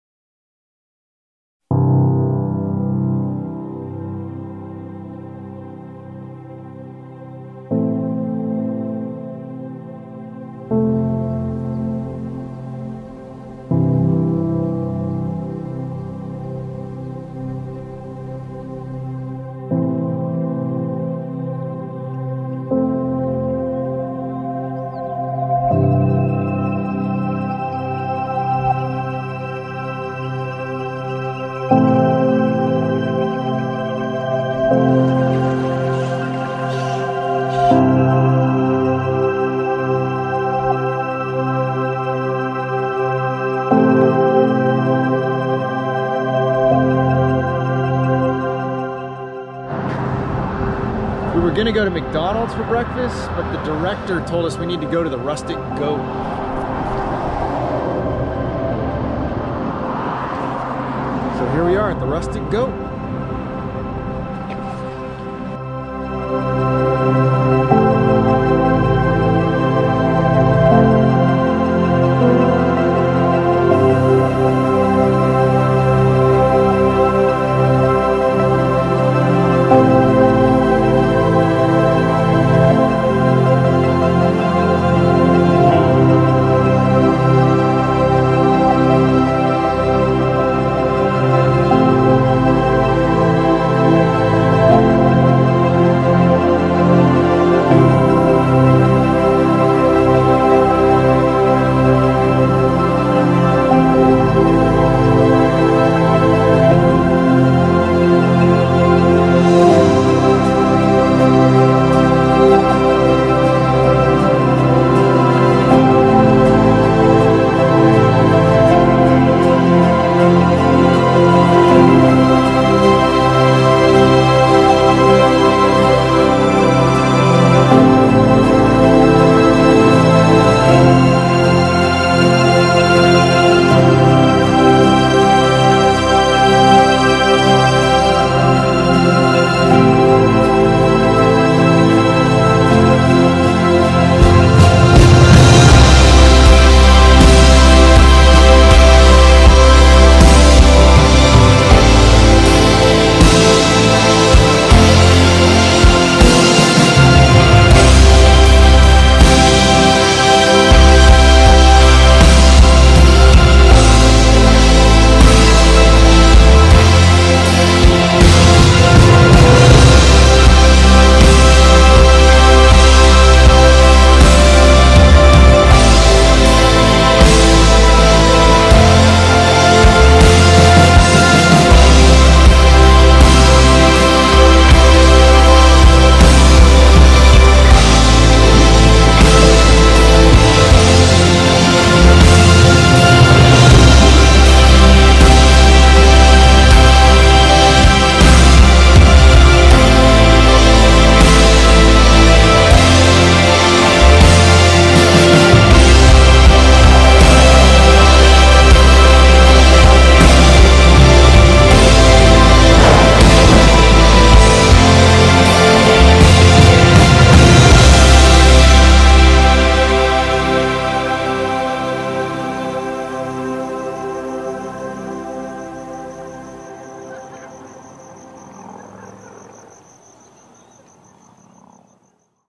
Alaska
A few clips of Alaska from GoPro Hero 7 and DJI Mavic Air Drone.